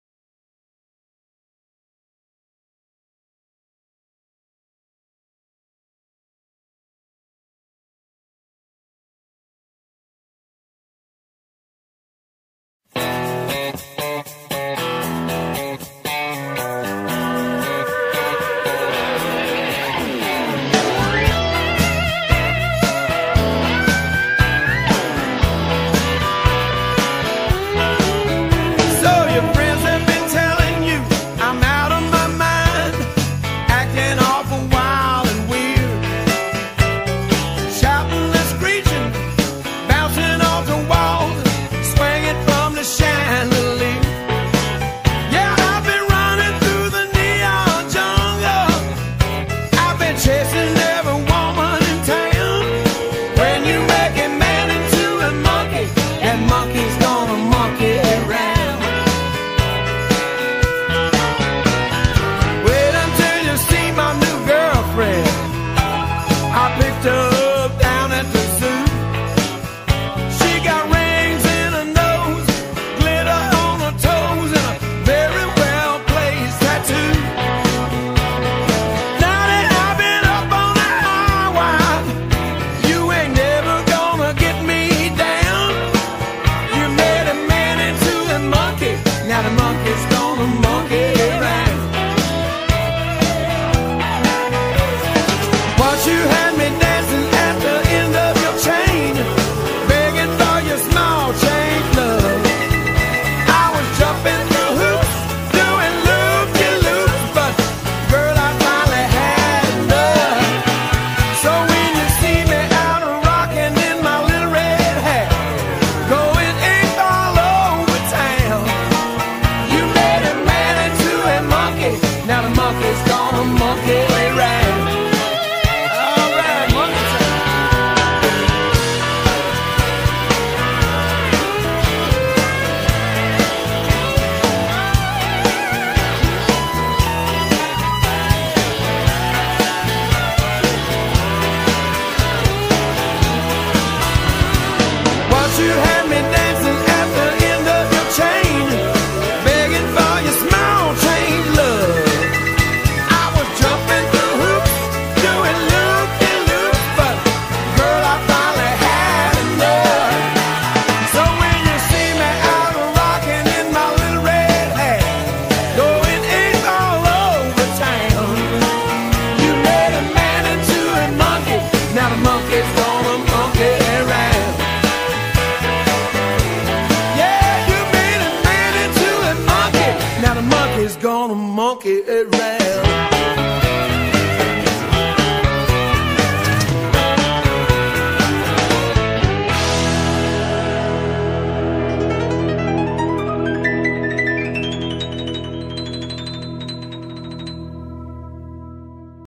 Begin to Swing - Line Dance - Choreo Larry Bass ( May 2019 )
32 Count - 4 Wall
Absolute Beginner Level Line Dance
Dance by IMA Line Dance (INA - Kota Harapan)